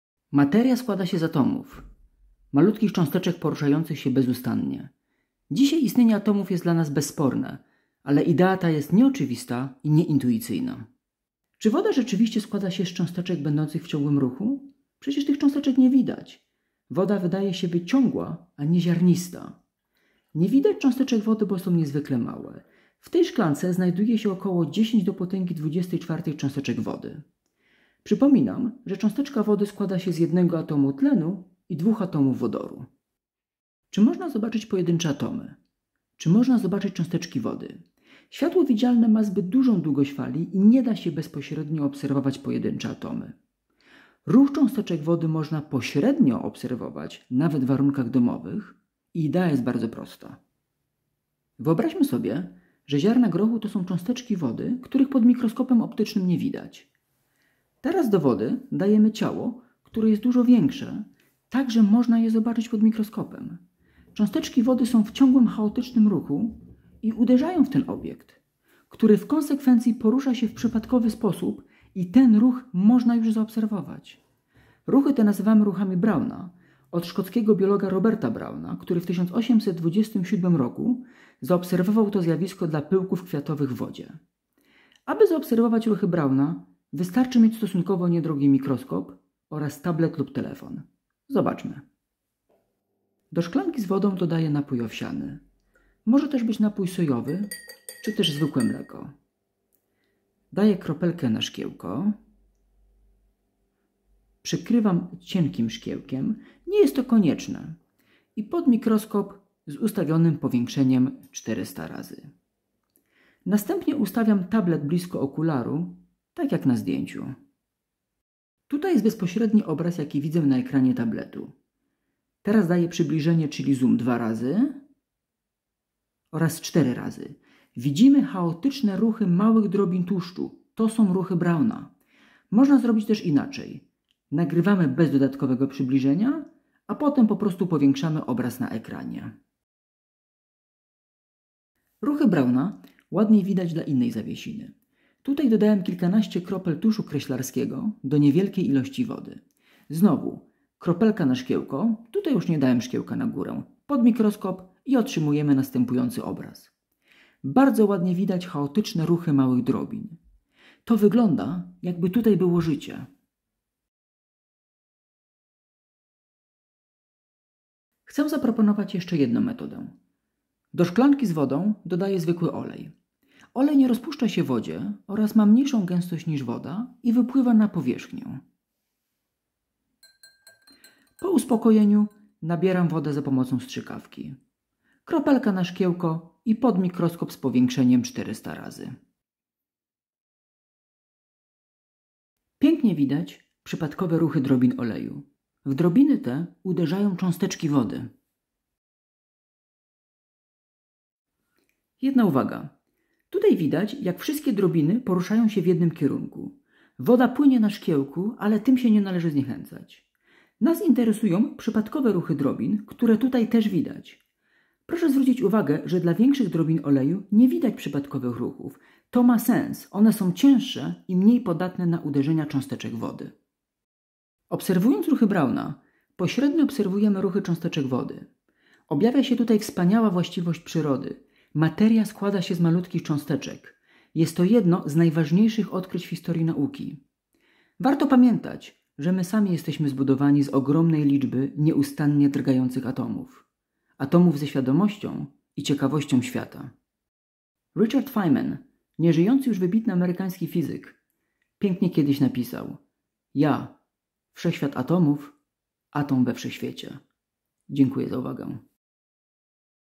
Atomy i ruchy Browna
Pokazuję jak łatwo można zaobserwować ruchy Browna. Ruchy Browna są manifestacją faktu, że  materia składa się z malutkich cząsteczek będących w ciągłym ruchu.
Dodatkowe uwagi:
1) Zjawisko ruchów Browna zostało w pełni zrozumiane dzięki teoretycznym pracom Alberta Einsteina i polskiego fizyka Mariana Smoluchowskiego
3) Światło widzialne ma zbyt dużą długość fali i nie da się zbudować mikroskopu optycznego, który pozwoliłby naszym oczom obserwować pojedyncze atomy. Atomy można obrazować za pomocą np. mikroskopów elektronowych, gdzie zamiast światła wykorzystuje się wiązkę elektronów
4) W szklance (początek filmu) znajduje się około 120 gramów wody. Masa jednej cząsteczki wody to około 3 razy 10^(-23) grama. Oznacza to, że w szklance znajduje się około 4 razy 10^24 cząsteczek wody.
5) Na zdjęciu w okolicach 0:35 znajduje się aktor Jim Hutton